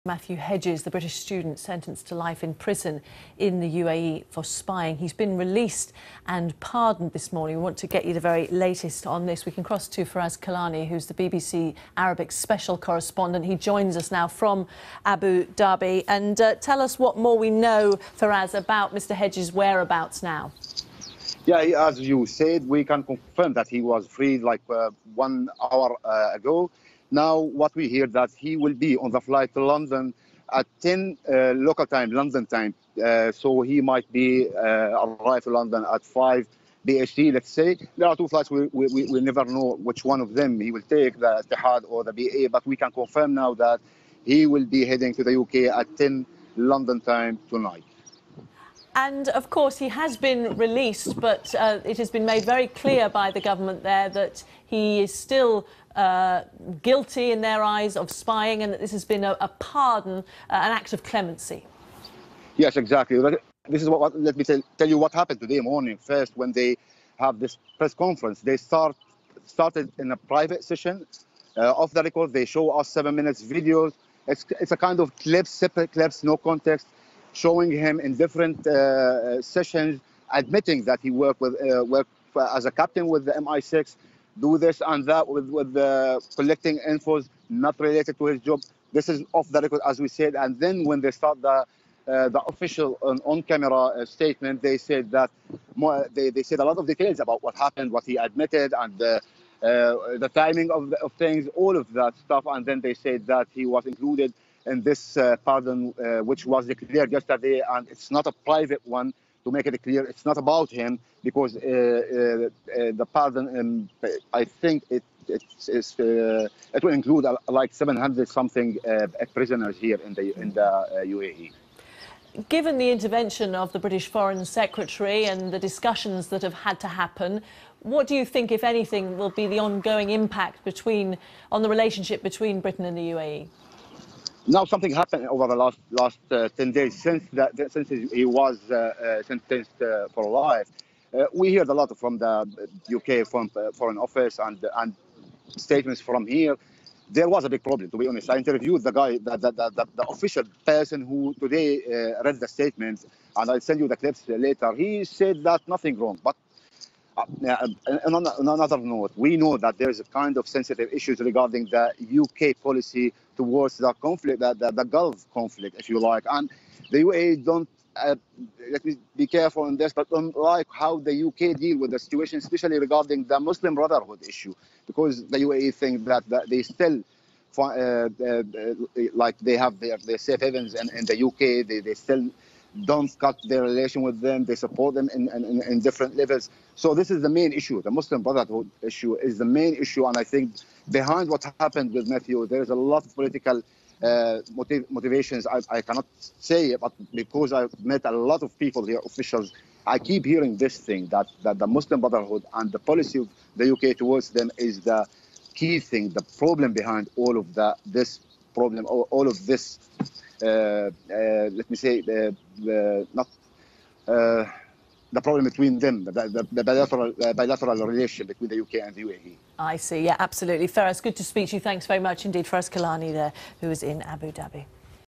Matthew Hedges: British academic pardoned by UAE - BBC News
A British academic who was jailed for spying in the United Arab Emirates has been pardoned.
Matthew Hedges, 31, denied spying and said he had been researching his PhD.
His wife, Daniela Tejada, who appealed for clemency, said she was "elated" and "can't wait to have him back home".
The UAE issued the pardon as part of a series of orders on the country's National Day anniversary. However, a spokesman said Mr Hedges was "100% a secret service operative".
BBC Arabic special correspondent Feras Kilani said Mr Hedges had been freed and was due to fly back to London tonight.

But a spokeswoman for Ms Tejada said: "He has not been released."